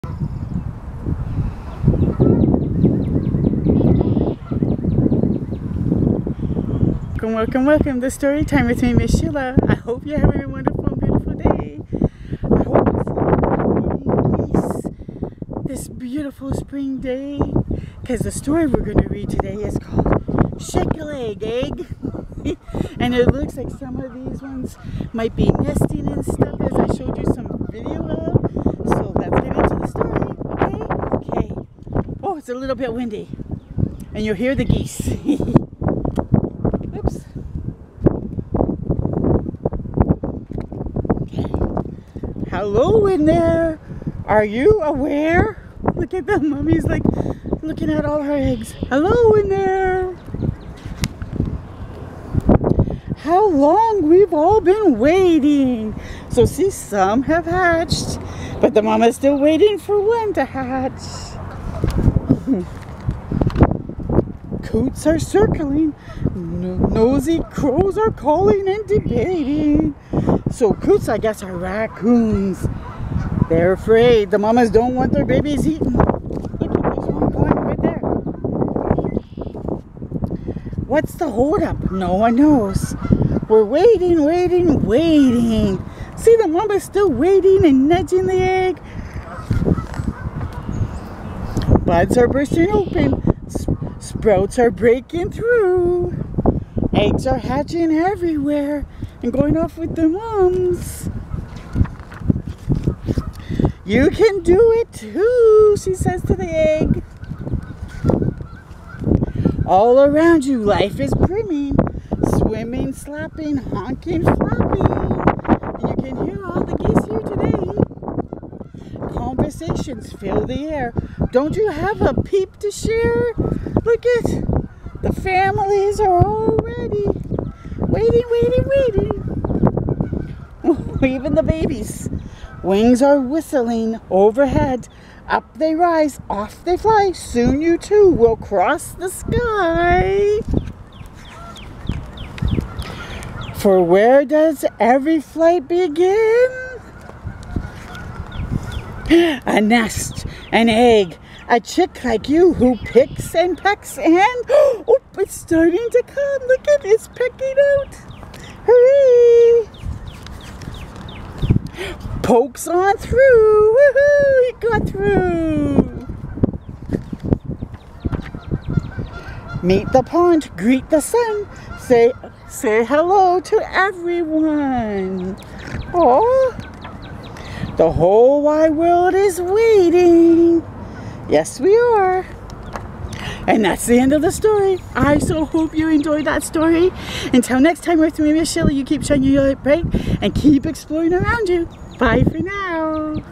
Shake A Leg, Egg!! Read Aloud
Kurt Cyrus delights us in the long anticipated wait for one last baby to arrive.